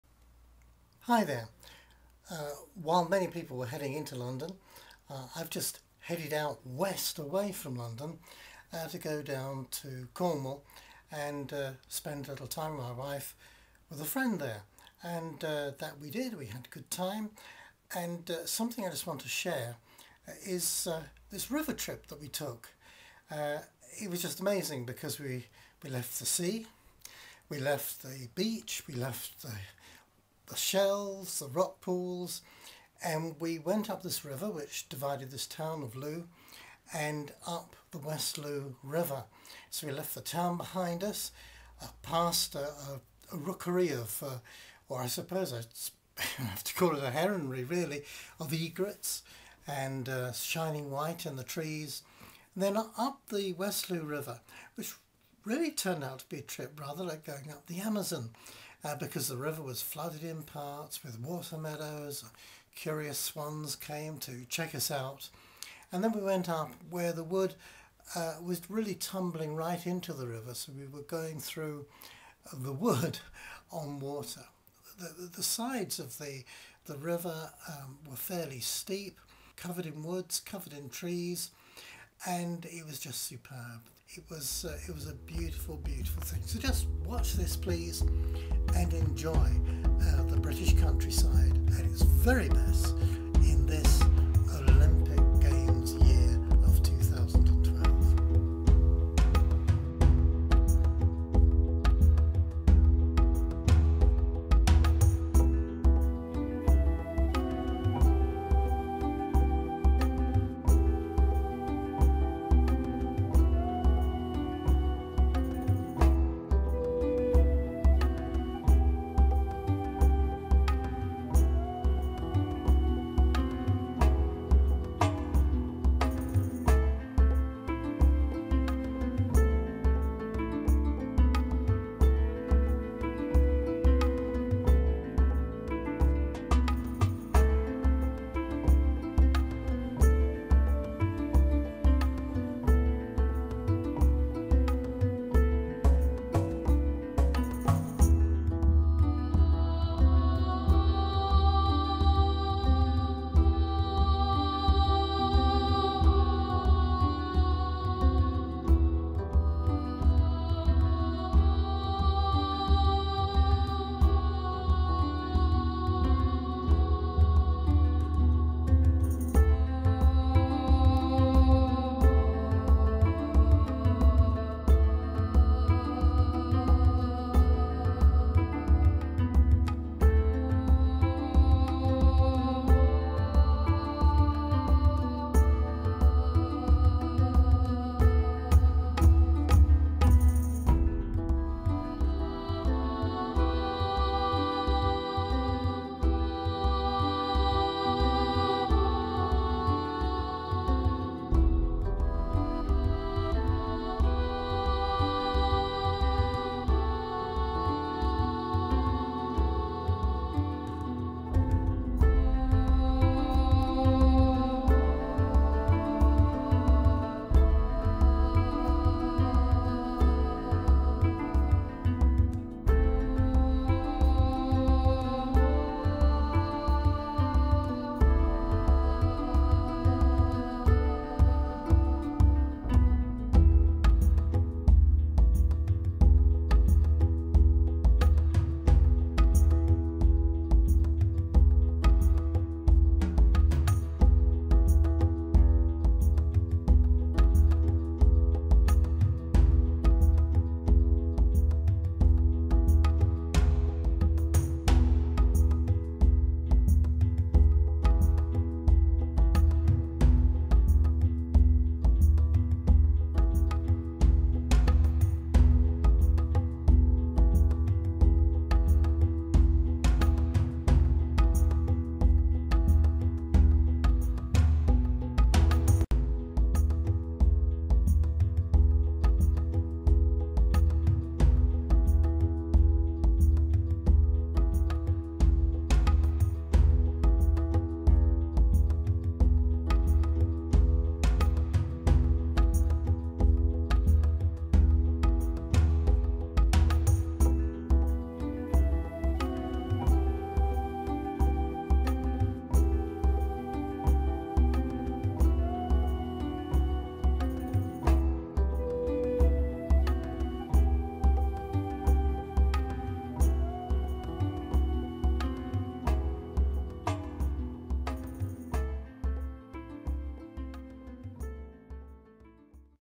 Sailing the West Looe River
From seaside to farmland via wooded hills tumbling into a flooded river and water meadows. English countryside at its finest as the elements merge into one during uncertain summer weather in Cornwall.